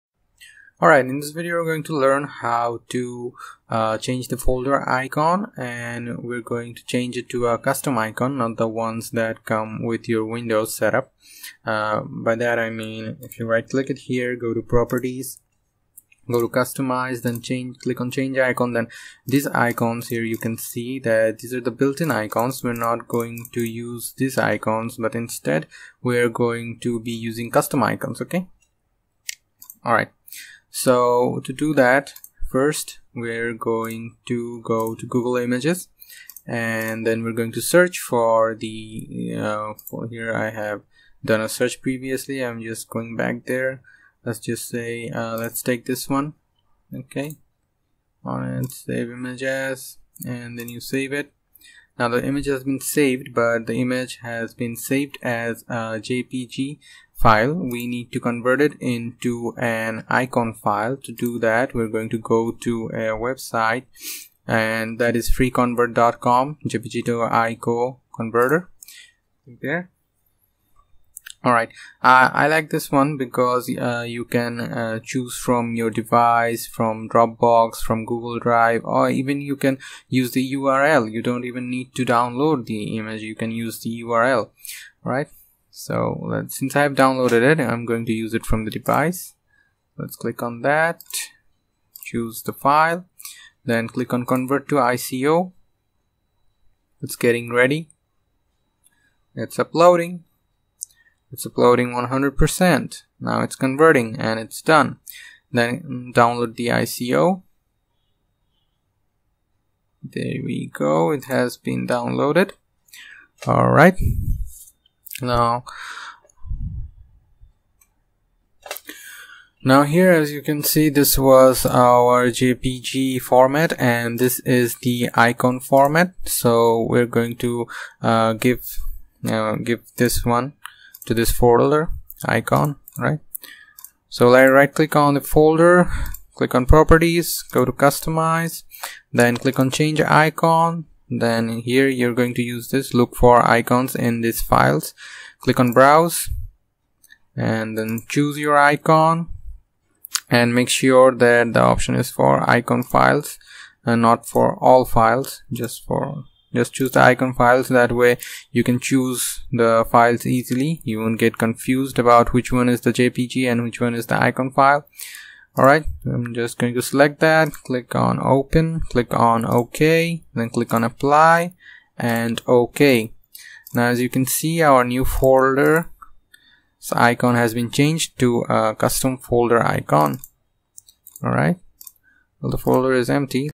How to change Folder Icon in Windows
In this video we are going to learn how we can make and apply custom folder icons !

Like my FB page to get updates and to participate in quizzes  & Quiz competitions